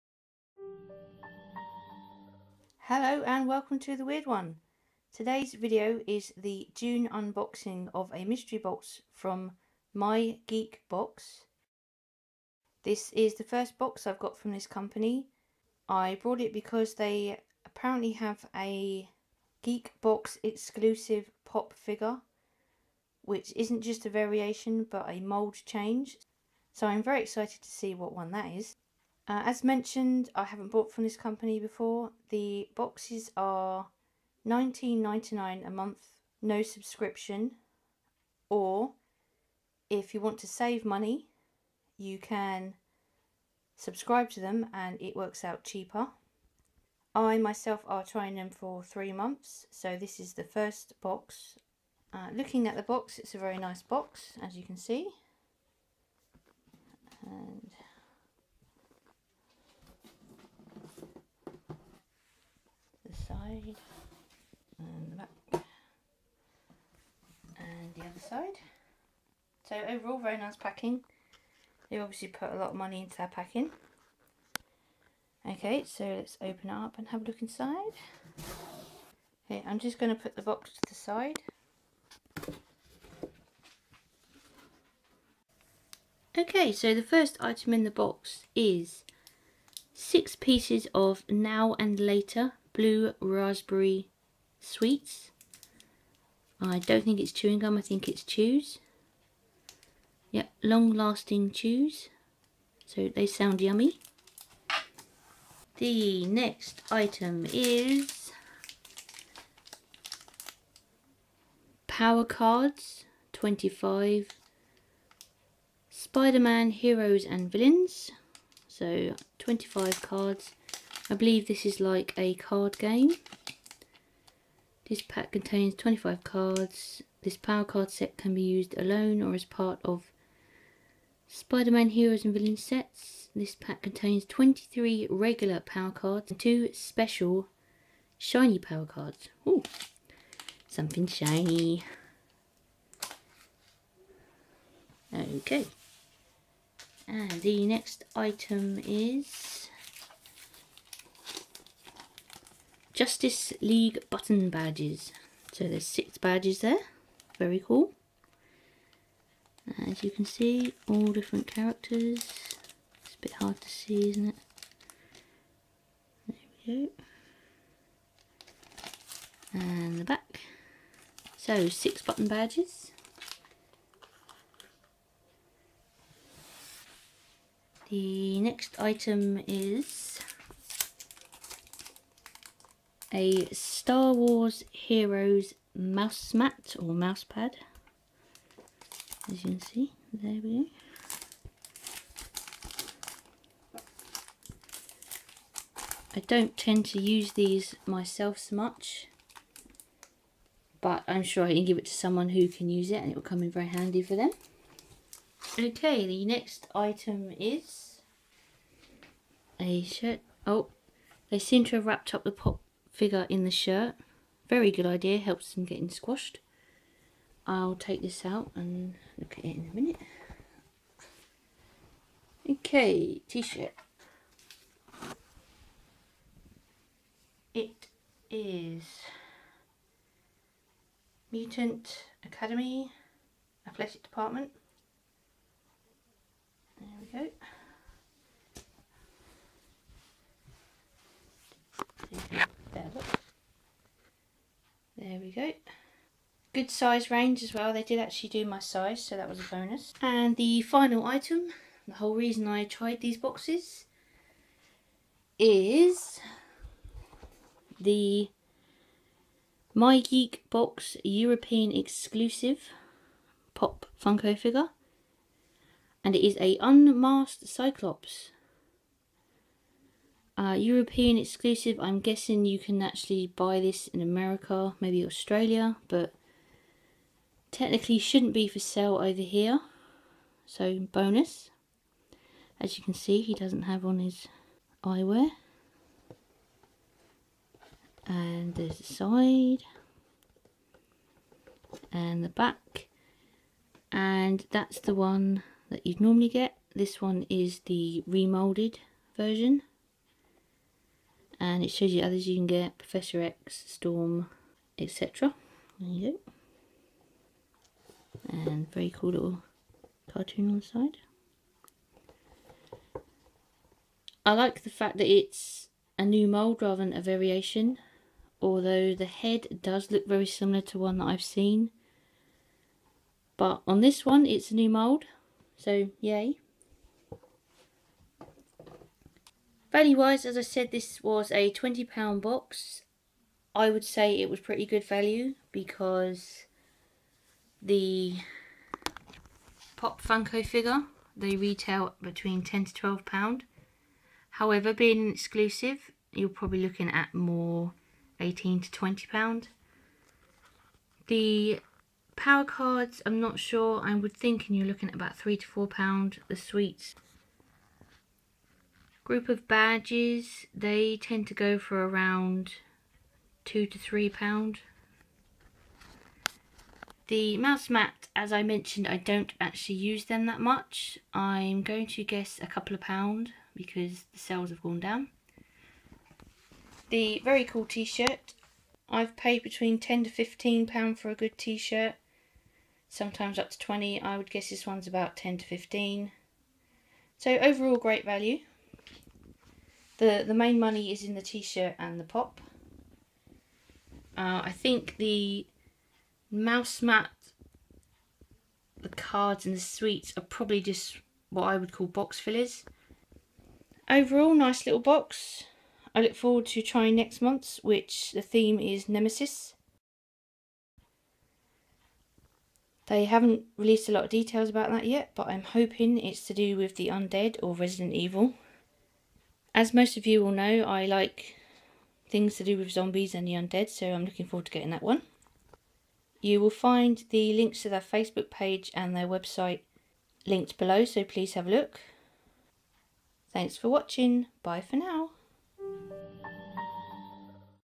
My Geek Box June mystery box UnBoxing: Abilities exclusive funko/pop - The Weird One - (UK)
My Geek Box mystery UnBoxing June theme: Abilities with exclusive funko/pop figure/bobble head :)